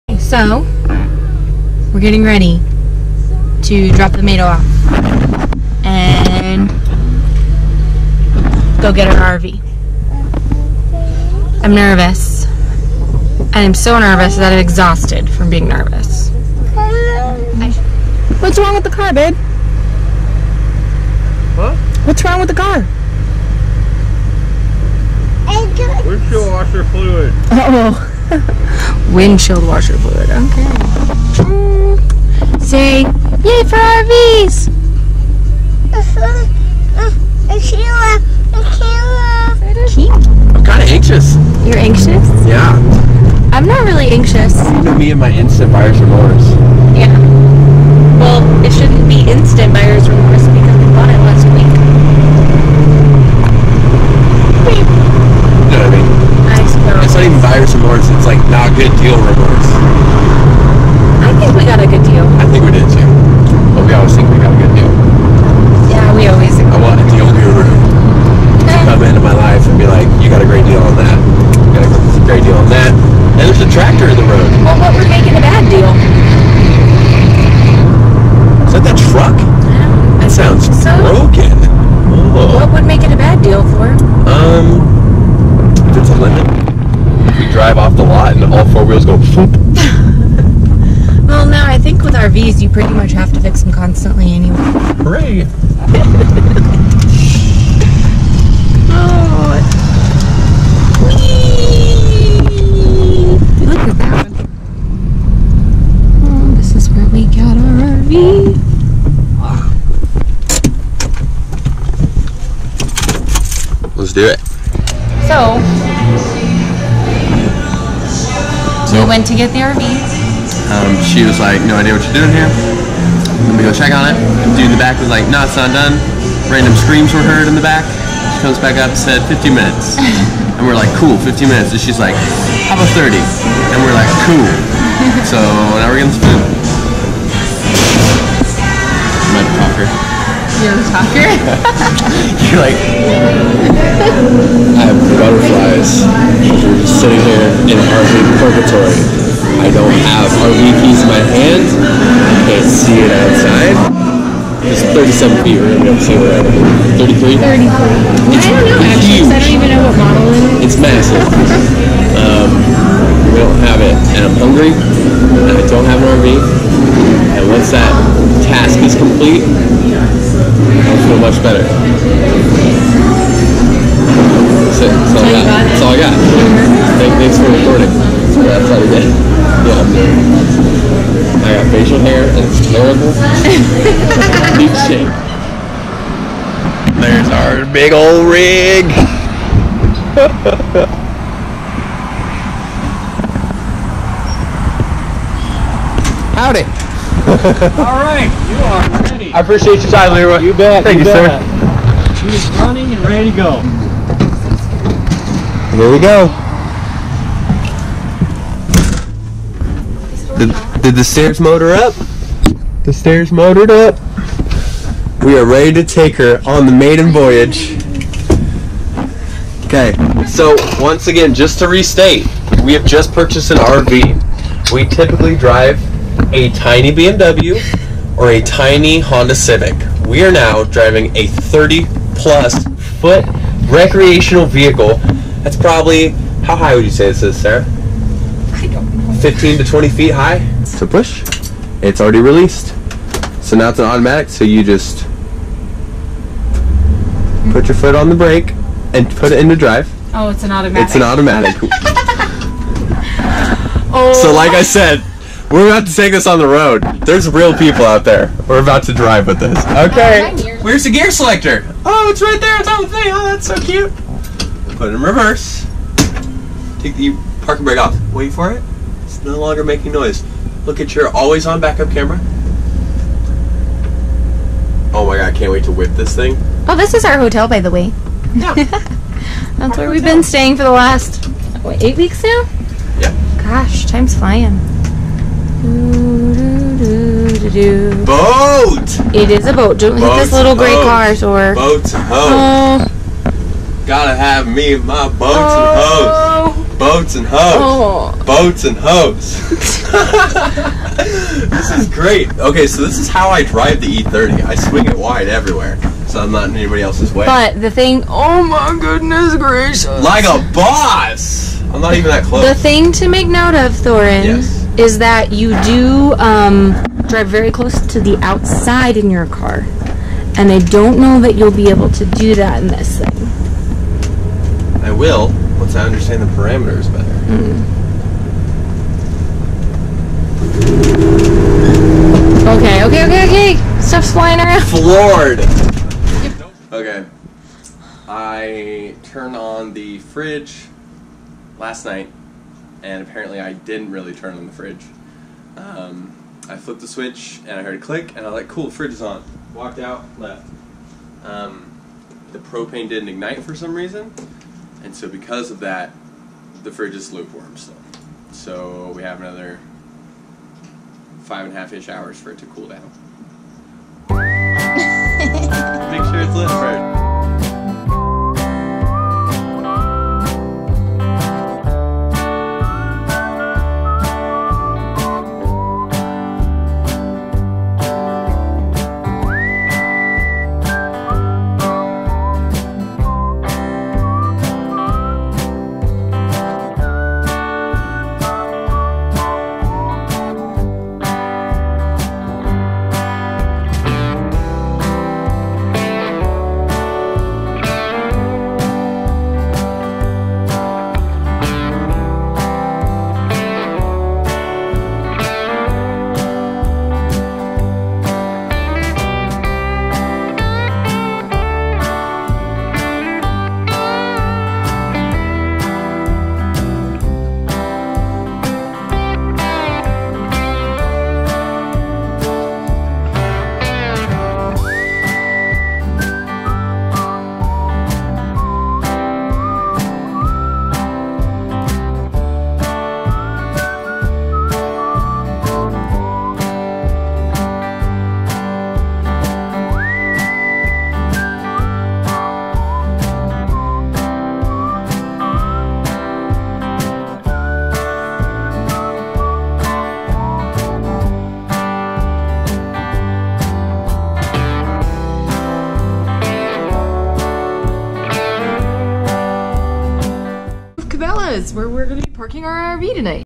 The Pick Up (EP 1)
Picking up our RV from the dealership and moving in.  In this episode we move from our extended stay hotel into our new home. A short glimpse of our RV naivety.